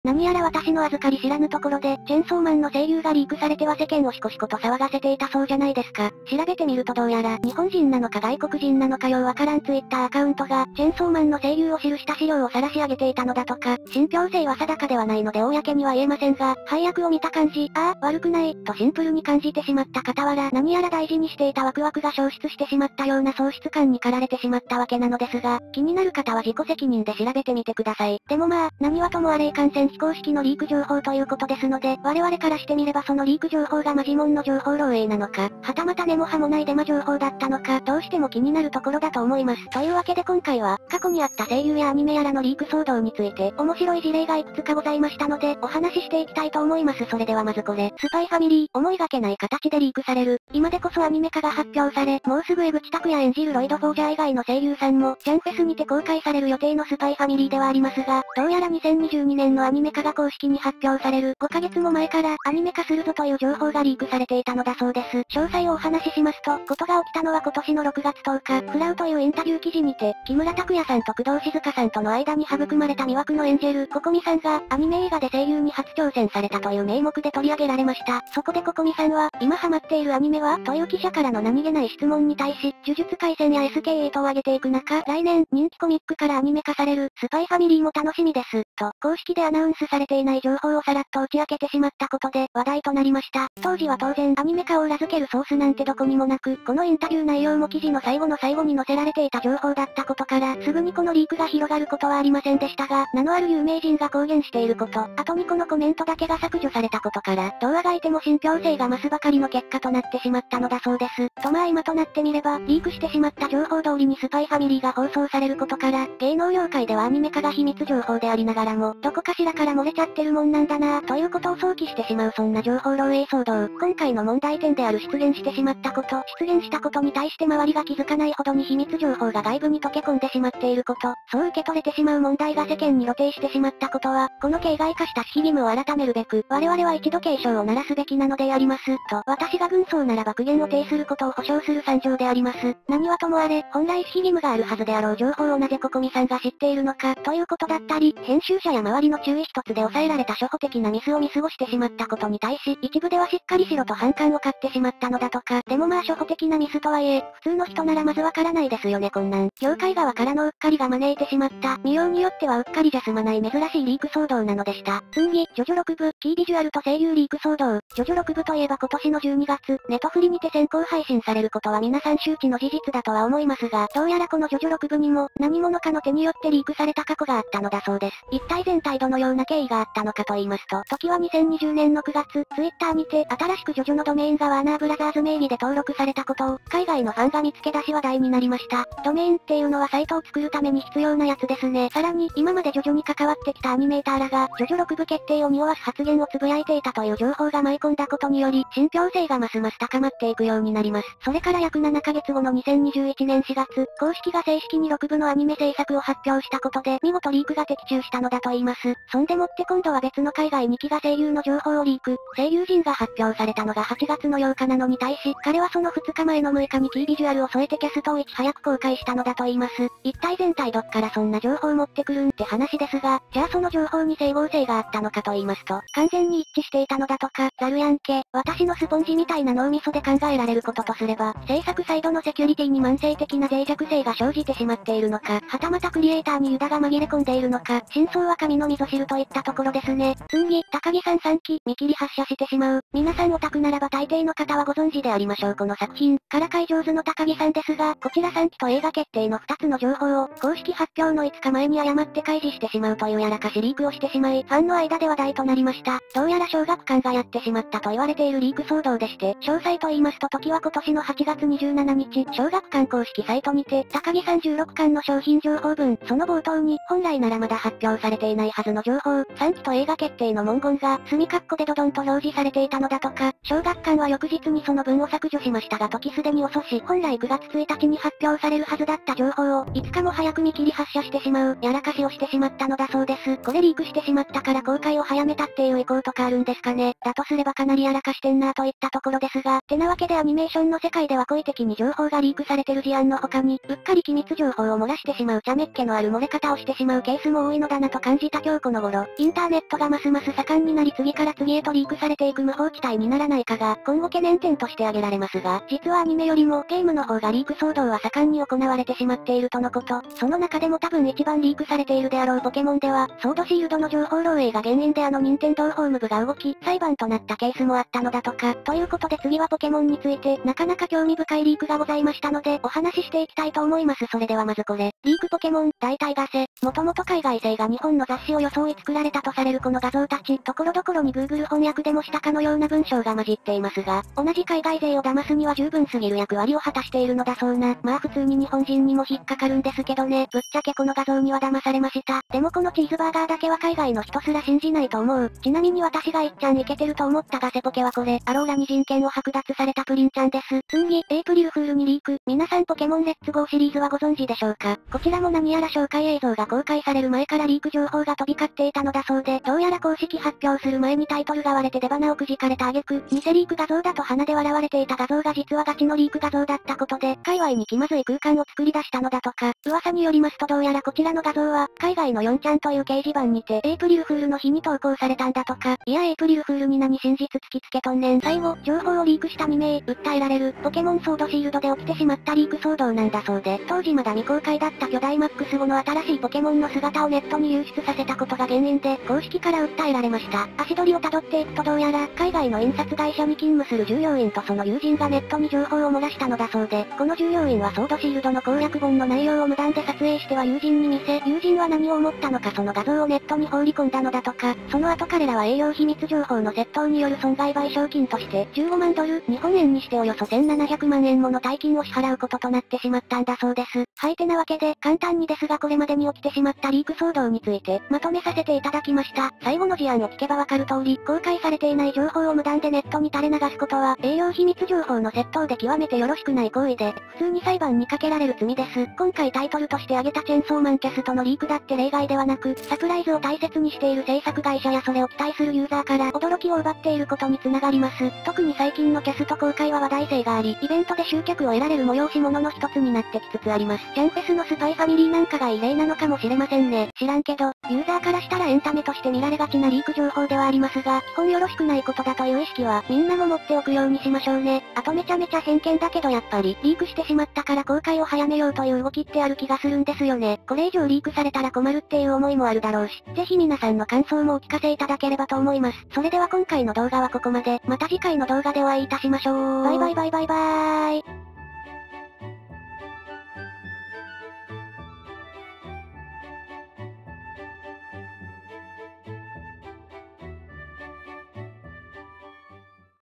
チェンソーマンの声優がリークされた!?有名芸能人がうっかり情報漏洩?!アニメオタクを騒がせたリーク騒動達。【ゆっくり】
これでガセだったら笑う。

お借りした画像、音楽等
いらすとや